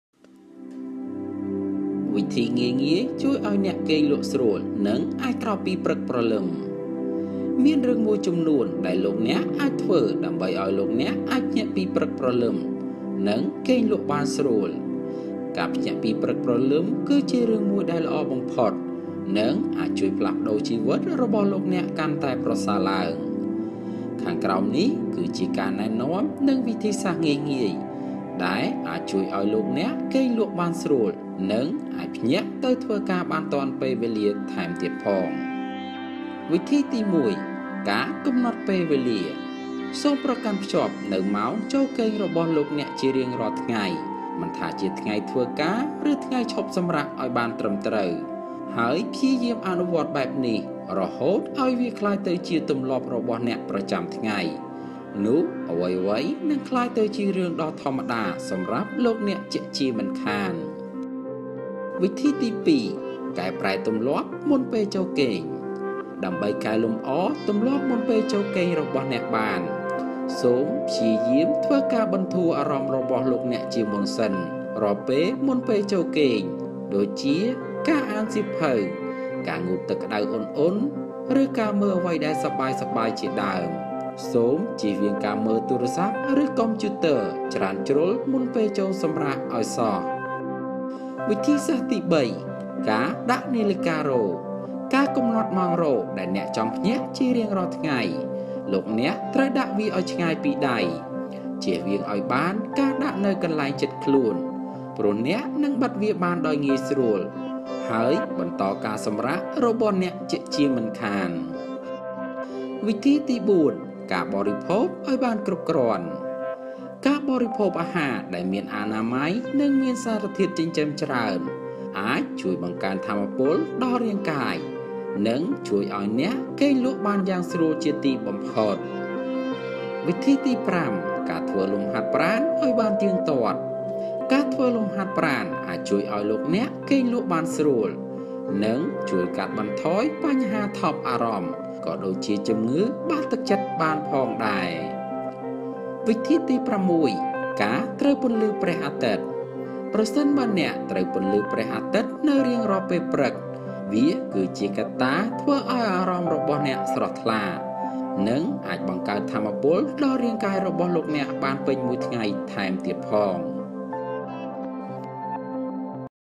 វិធីងាយៗជួយអោយអ្នកគេងលក់ស្រួល និងអាចក្រោកពីព្រឹកព្រលឹម [សហគំនិត]
មានរឿងមួយចំនួនដែលអ្នកអាចធ្វើដើម្បីជួយឱ្យអ្នកអាចភ្ញាក់ពីព្រឹកព្រលឹម និងគេងលក់បានស្រួល។ ការភ្ញាក់ពីព្រលឹមគឺជារឿងមួយដែលល្អបំផុតនិងអាចជួយផ្លាស់ប្តូរជីវិតរបស់អ្នកអោយកាន់តែល្អប្រសើរឡើង។ខាងក្រោមនេះជាការណែនាំនិងវិធីព្យាបាលងាយៗដែលអាចជួយឱ្យអ្នកងលក់ស្រួលនិងអាចភ្ញាក់ទៅធ្វើការបានទាន់ពេលវេលាថែមទៀតផង៖